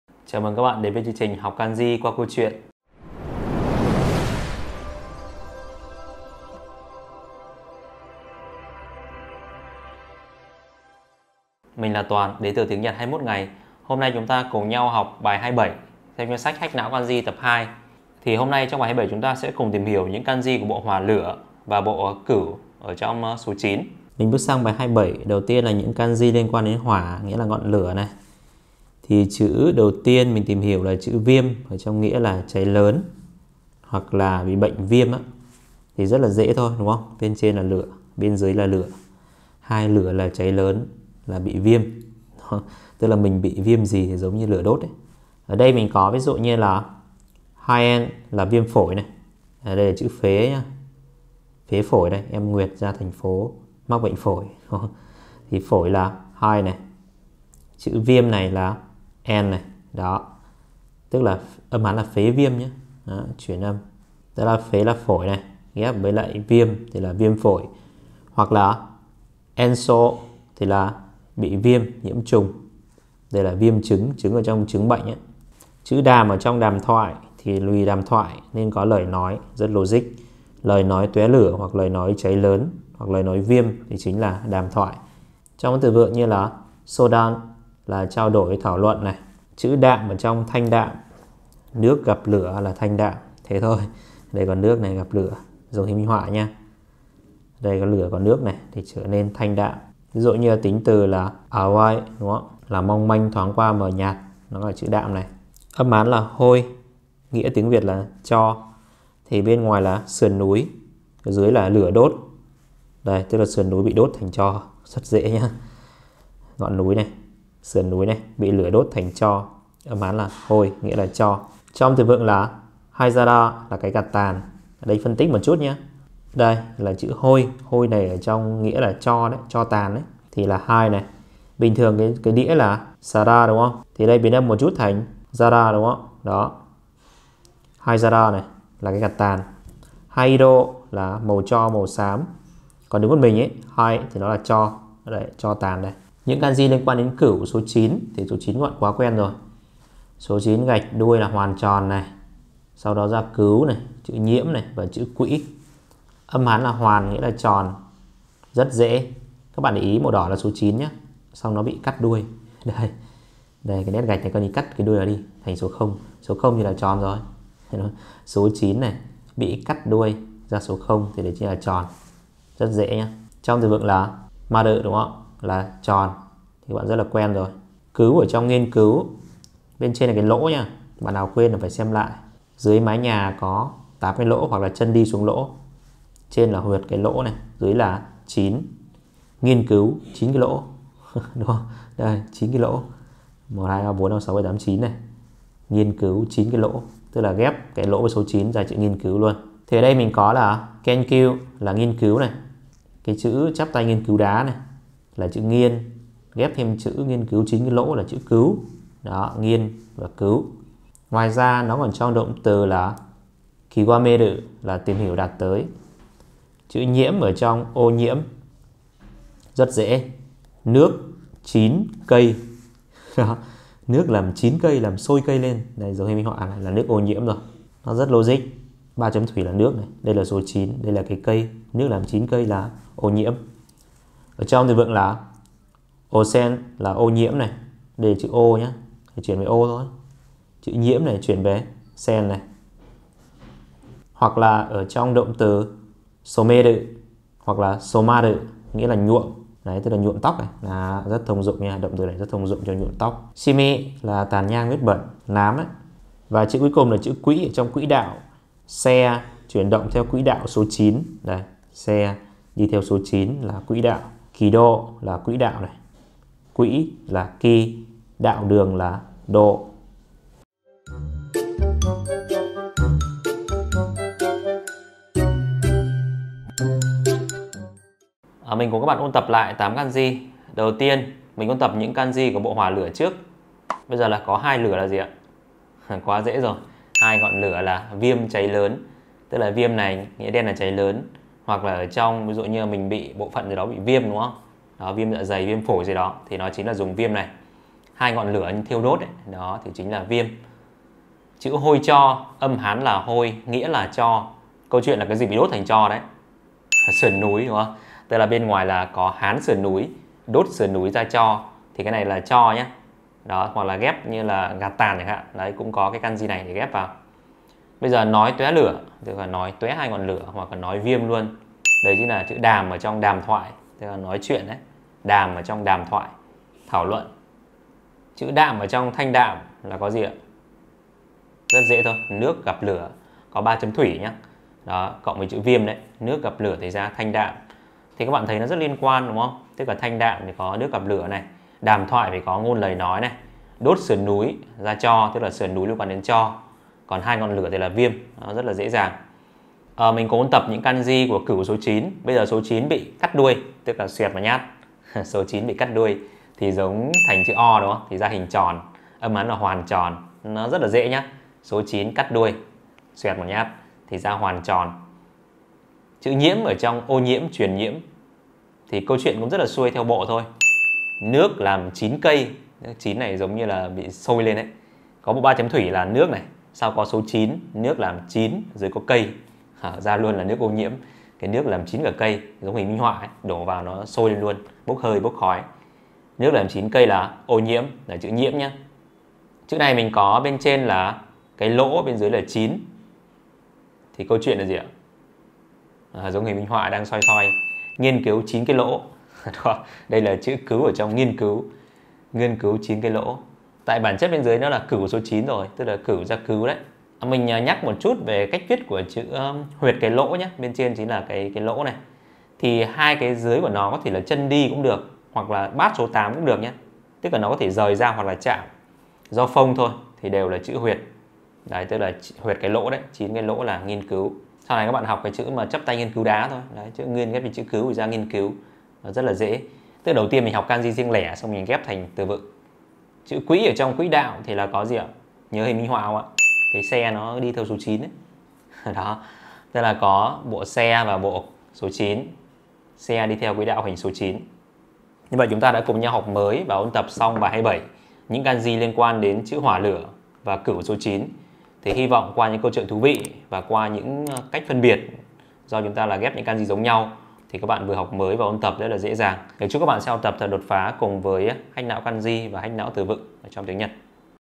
[Hack Não 2136 Kanji 2] Bài 27: SƯỜN NÚI (厂) bị LỬA (火) đốt thành TRO (灰)
Tiếng Nhật 21 Ngày xin gửi đến bạn Bài học số 27 trong chuỗi video hướng dẫn học kanji QUA CÂU CHUYỆN VÀ BỘ THỦ GIỐNG NHAU của sách ''Hack Não 2136 Kanji Tập 2''.
★ Đặt mua sách từ TỦ SÁCH HACK NÃO TIẾNG NHẬT ★